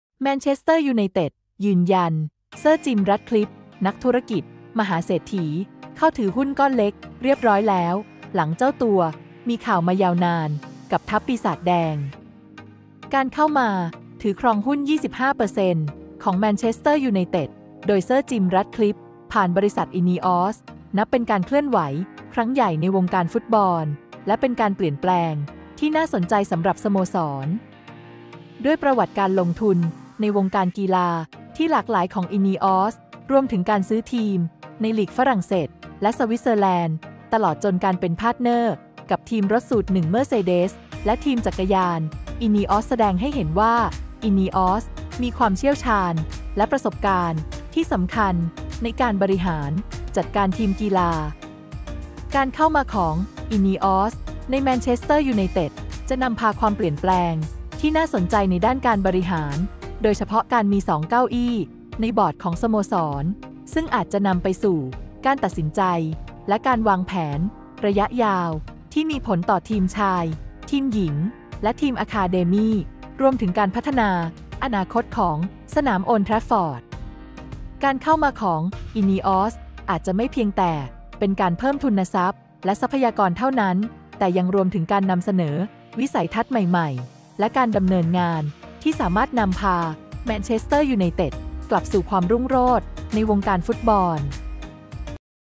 มาแล้วอย่างทางการ! เซอร์ จิม แรตคลิฟฟ์ ร่วมถือหุ้นเจ้าของ ผีแดง
แมนเชสเตอร์ ยูไนเต็ด ยืนยัน  เซอร์ จิม แรตคลิฟฟ์ นักธุรกิจมหาเศรษฐี เข้าถือหุ้นก้อนเล็กเรียบร้อยแล้ว หลังเจ้าตัวมีข่าวมายาวนานกับทัพ "ปีศาจแดง"

ข่าวกีฬาวันนี้ รายงานว่า การเข้ามาถือครองหุ้น 25% ของแมนเชสเตอร์ ยูไนเต็ด โดยเซอร์ จิม แรตคลิฟฟ์ ผ่านบริษัท INEOS นับเป็นการเคลื่อนไหวครั้งใหญ่ในวงการฟุตบอล และเป็นการเปลี่ยนแปลงที่น่าสนใจสำหรับสโมสร
ด้วยประวัติการลงทุนในวงการกีฬาที่หลากหลายของ INEOS รวมถึงการซื้อทีมในลีกฝรั่งเศสและสวิสเซอร์แลนด์ ตลอดจนการเป็นพาร์ทเนอร์กับทีมรถสูตรหนึ่ง เมอร์เซเดส และทีมจักรยาน Ineos Grenadiers แสดงให้เห็นว่า INEOS มีความเชี่ยวชาญและประสบการณ์ที่สำคัญในการบริหารจัดการทีมกีฬา
การเข้ามาของ INEOS ในแมนเชสเตอร์ ยูไนเต็ด จะนำพาความเปลี่ยนแปลงที่น่าสนใจในด้านการบริหาร โดยเฉพาะการมีสองเก้าอี้ในบอร์ดของสโมสร ซึ่งอาจจะนำไปสู่การตัดสินใจและการวางแผนระยะยาวที่มีผลต่อทีมชาย, ทีมหญิง และทีมอะคาเดมี่ รวมถึงการพัฒนาอนาคตของสนามโอลด์ แทรฟฟอร์ด
การเข้ามาของ INEOS อาจจะไม่เพียงแต่เป็นการเพิ่มทุนทรัพย์และทรัพยากรเท่านั้น แต่ยังรวมถึงการนำเสนอวิสัยทัศน์ใหม่ ๆ และการดำเนินงานที่สามารถนำพาแมนเชสเตอร์ ยูไนเต็ด กลับสู่ความรุ่งโรจน์ในวงการฟุตบอล